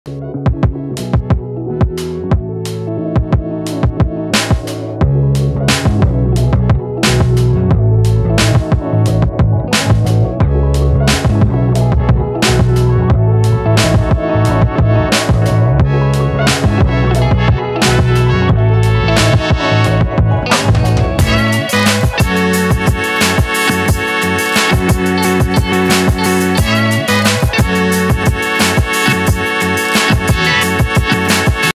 new latest design maang tikka | sila jewellery design
hello frienrs, i am sila, today i showing gold tikli design, you are watching sila jewellery design, this design is very good looking gold jewellery tikli design, i hope you like this design, if you want to know anything, please comment, Subscribe for more beautiful jewelry designs, thanks for watching.